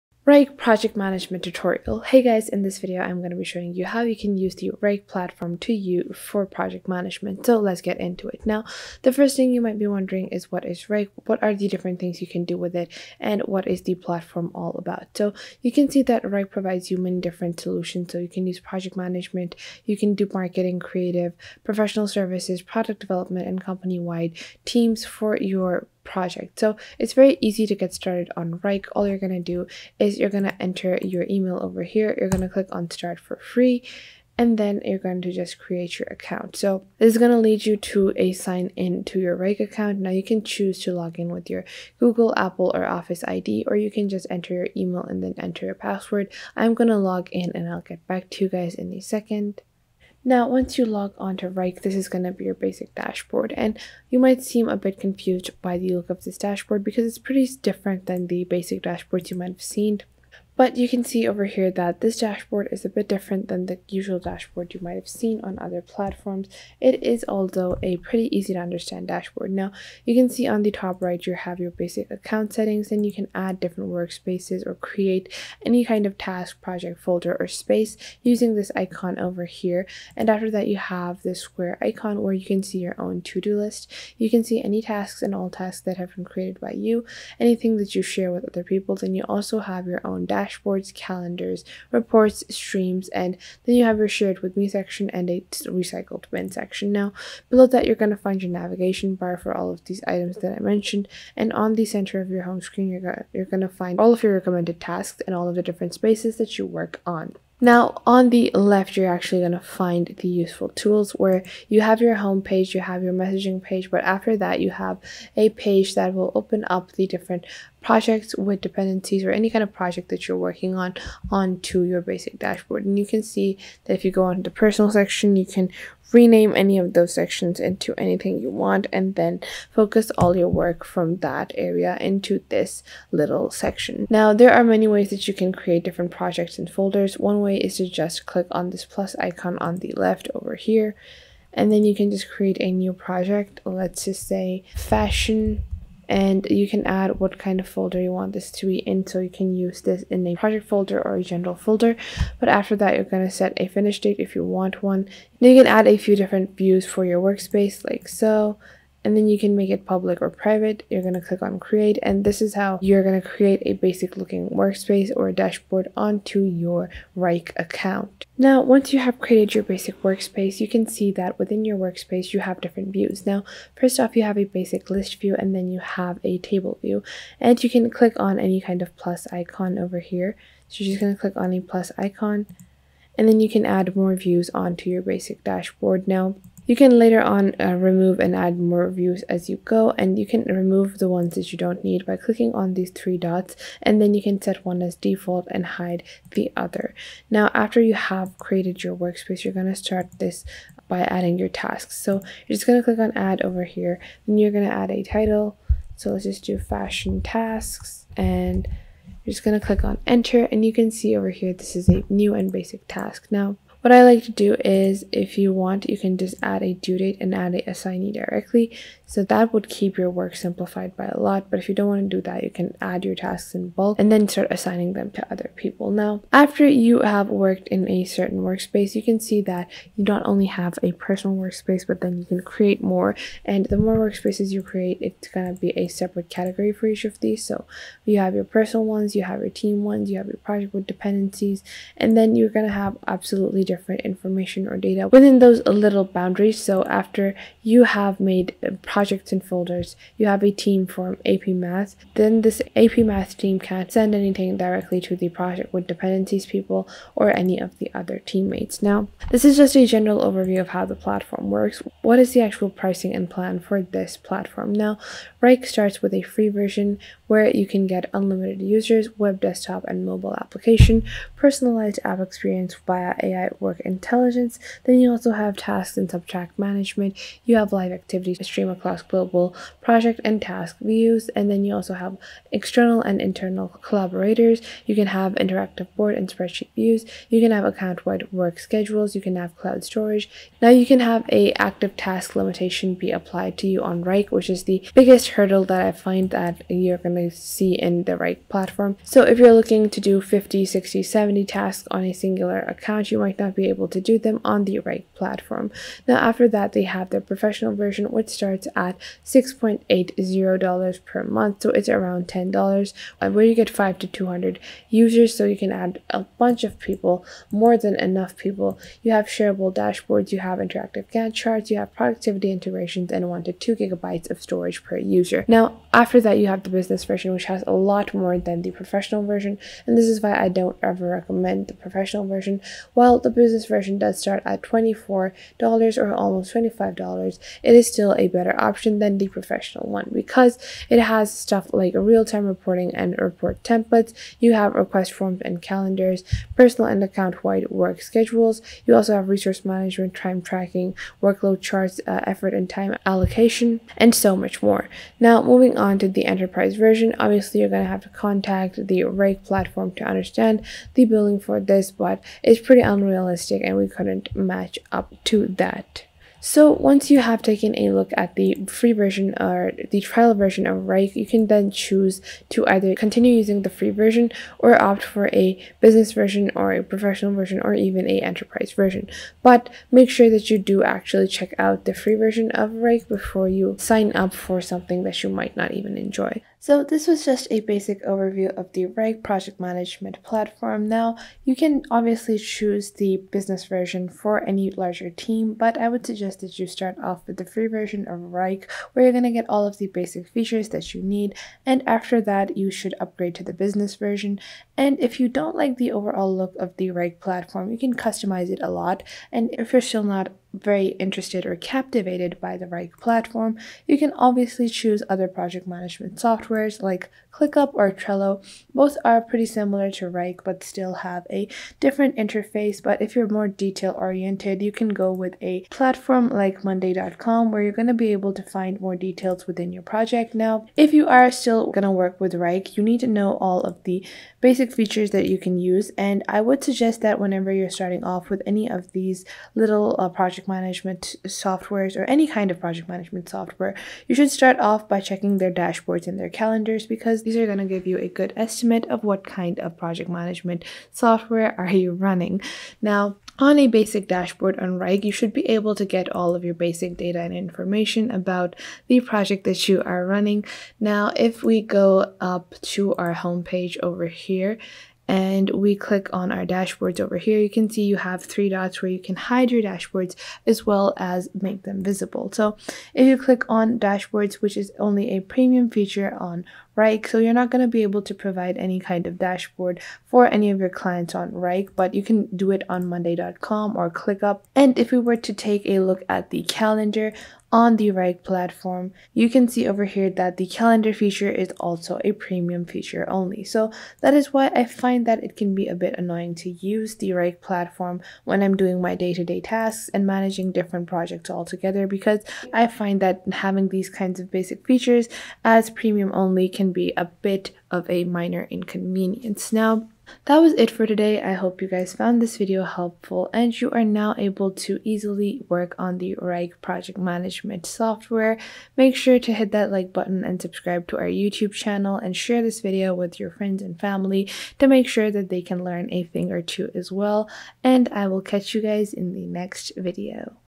Wrike Project Management 2022 | How to Use Wrike for Project Management
In this video I show you How to Use Wrike Project management to manage your tasks and projects efficiently and what features Wrike project management software offers to meet your ultimate work needs.